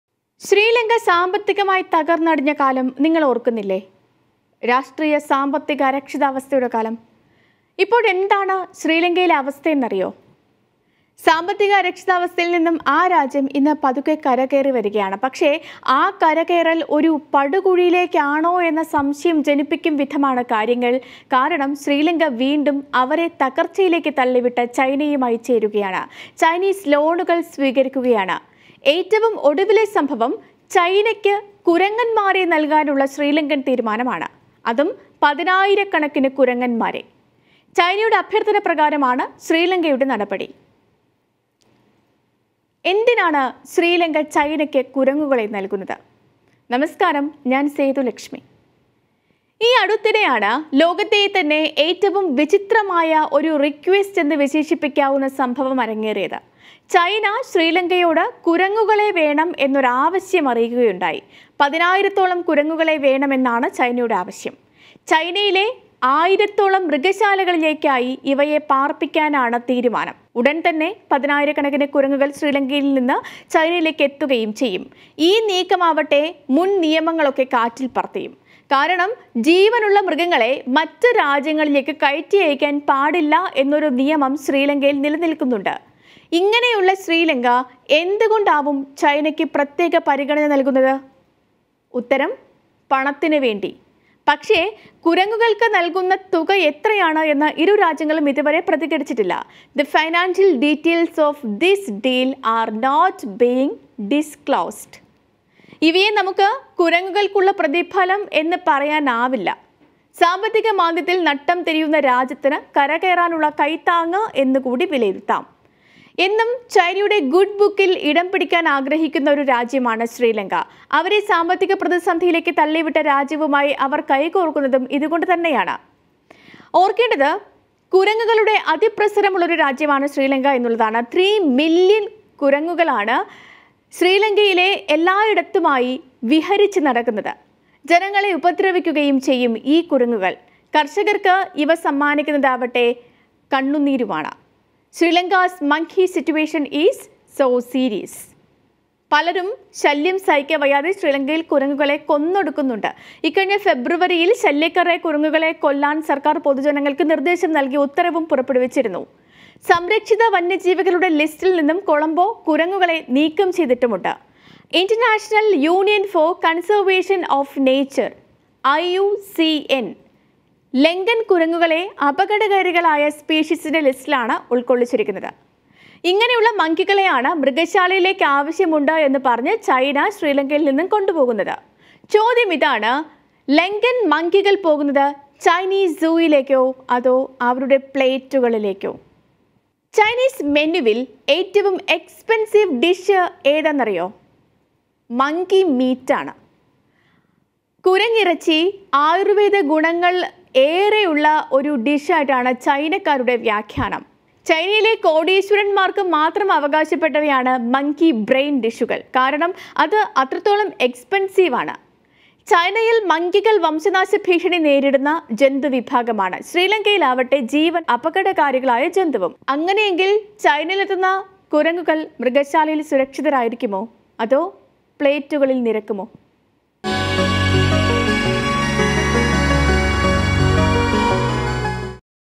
ചൈനയ്ക്ക് എന്തിന് ശ്രീലങ്കൻ കുരങ്ങുകൾ? ആ ലക്ഷ്യം ഇത്‌ | Sri Lanka | Monkey
ശ്രീലങ്ക സാമ്പത്തിക തകര്‍ന്നടിഞ്ഞത് നിങ്ങള്‍ ഓര്‍ക്കുന്നില്ലേ? ഇപ്പോള്‍ എന്താണ് ശ്രീലങ്കയിലെ അവസ്ഥ എന്ന് അറിയോ?
സാമ്പത്തിക അരക്ഷിതാവസ്ഥയില്‍ നിന്ന് ആ രാജ്യം ഇന്ന് പതുക്കെ കരകയറുന്നു. പക്ഷേ ആ കരകയറല്‍ ഒരു പടുകുഴിയിലേക്ക് ആണോ എന്ന സംശയം ജനിക്കുക ആണ്. കാരണം, ശ്രീലങ്ക വീണ്ടും അവരെ തകര്‍ച്ചയിലേക്ക് തള്ളിവിട്ട ചൈനയുമായി ചേരുക ആണ്. ചൈനീസ് ലോണുകള്‍ സ്വീകരിക്കുക ആണ്. ഏറ്റവും ഒടുവിലെ സംഭവം- ചൈനയ്ക്ക് കുരങ്ങന്മാരെ നല്‍കാനുള്ള ശ്രീലങ്കന്‍ തീരുമാനം ആണ്. അതും 10,000 കുരങ്ങന്മാരെ. ചൈനയുടെ അഭ്യര്‍ത്ഥന പ്രകാരമാണ് ശ്രീലങ്കന്‍ നടപടി. എന്തിനാണ് ശ്രീലങ്ക ചൈനയ്ക്ക് കുരങ്ങുകളെ നല്‍കുന്നത്?